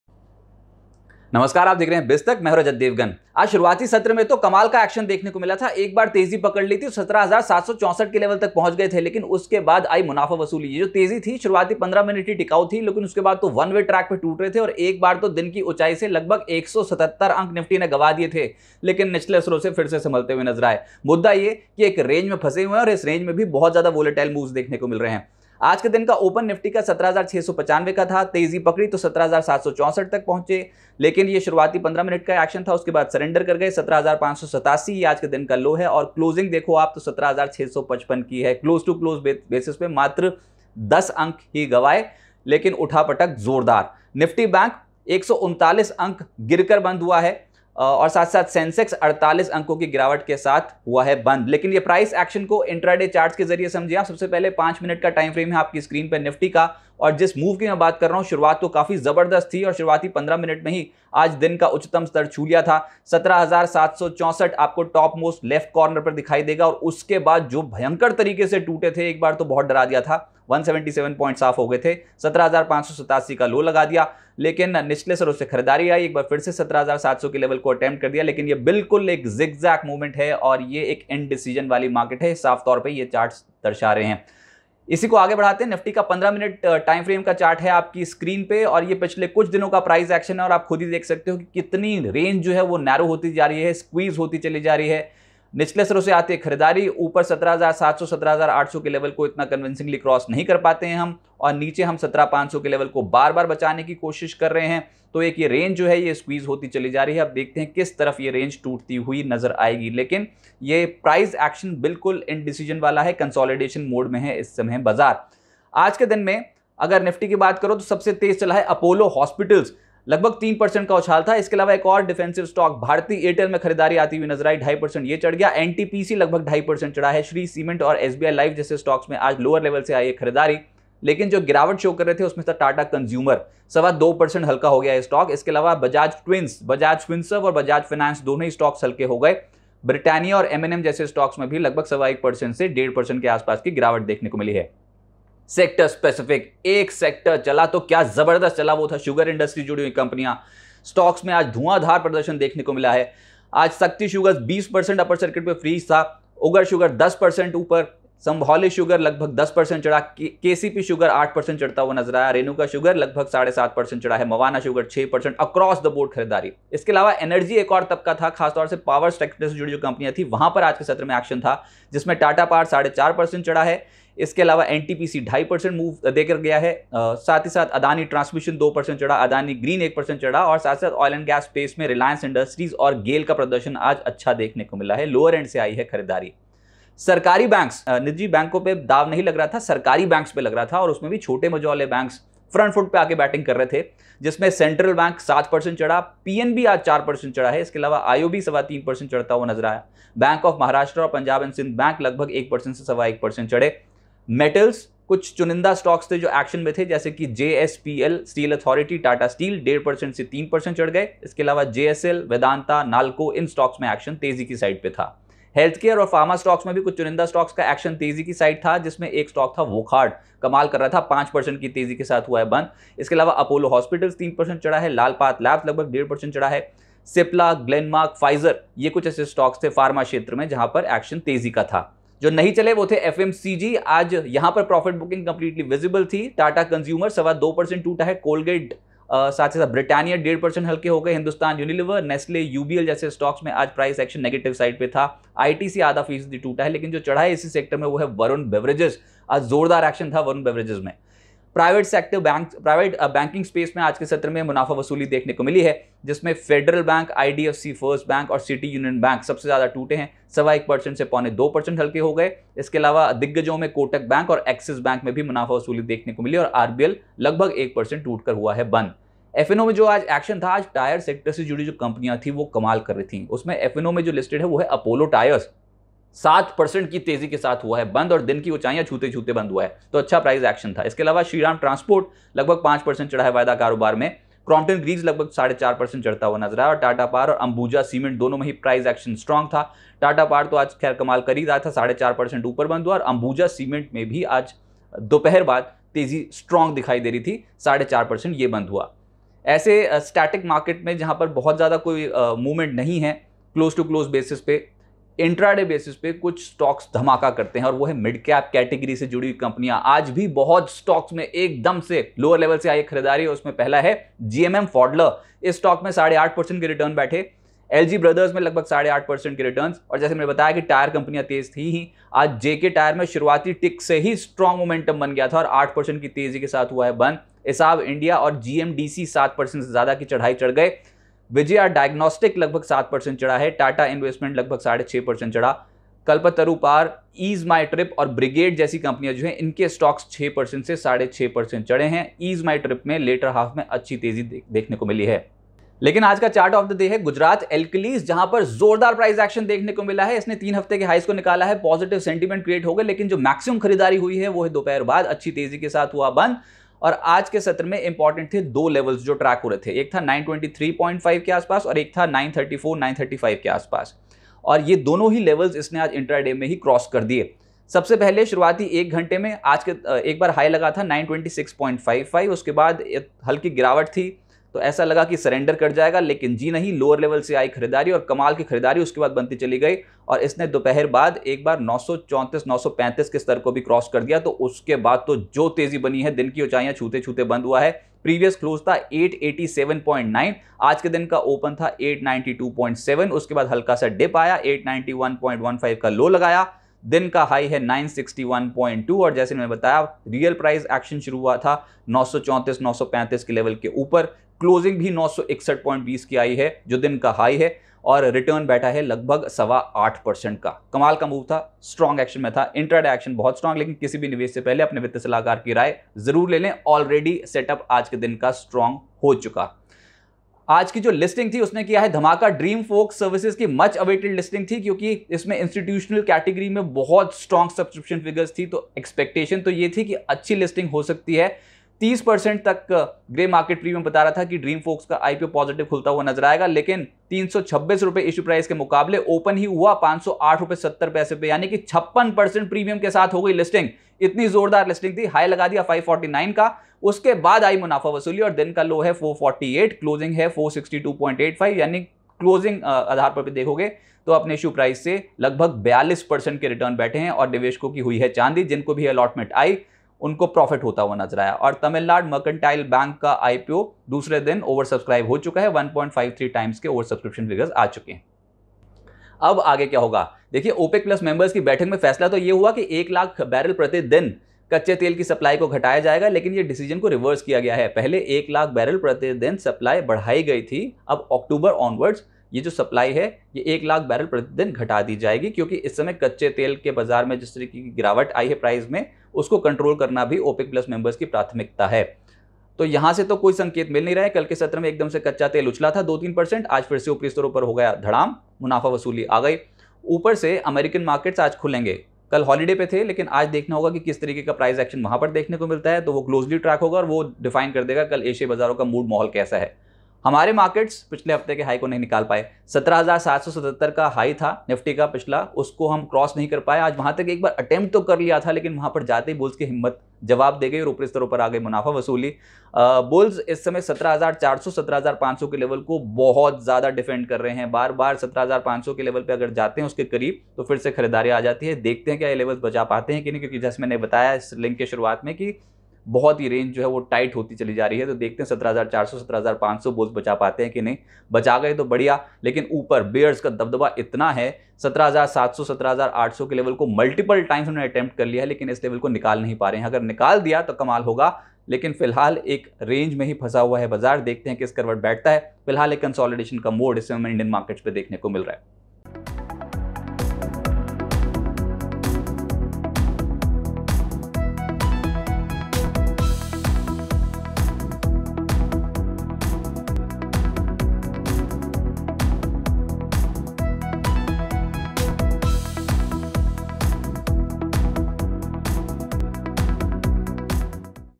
Share Bazar की सबसे बड़ी रुकावट ! |BIZTak
Sensex ends in the red, Nifty around 17,650; oil & gas, power, metals top gainers, financials drag